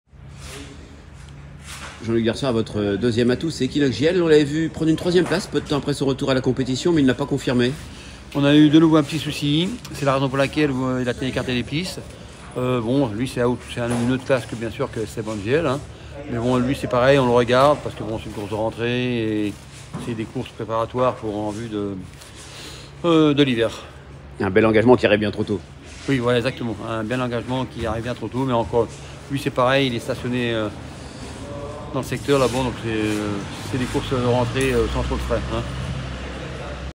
Jean-Luc Dersoir, entraîneur de Esteban Jiel (07/10 à Caen)
Jean-Luc Dersoir, entraîneur de Esteban Jiel (15) nous parle de ses chances dans le Quinté+, le Prix de la Ville de Caen sur l’hippodrome de Caen le 07/10/2023.